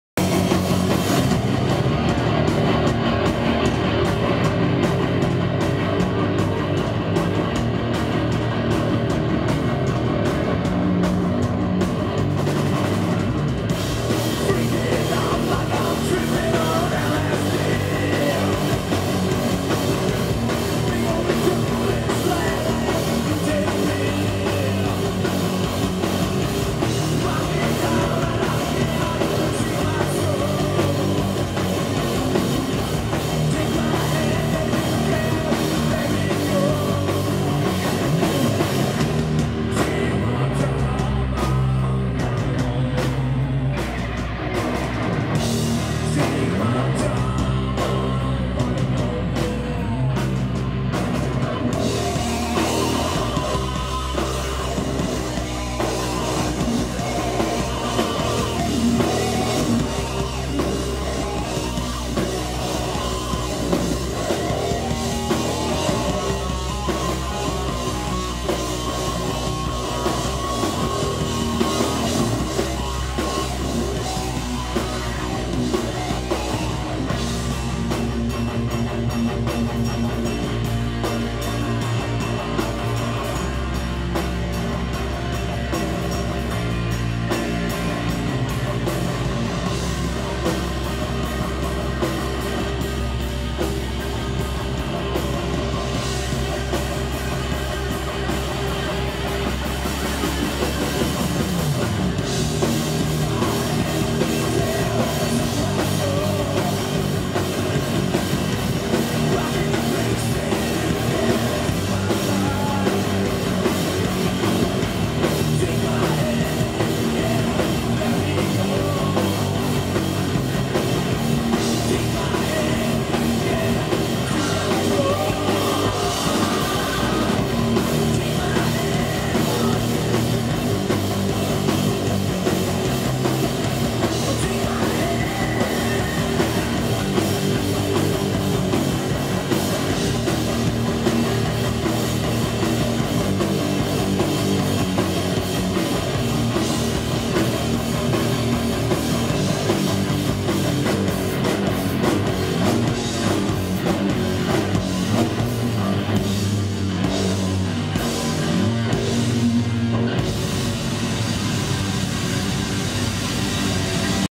Sigiriya at Roadburn 2012
A minute or so of killer stoner metal at Roadburn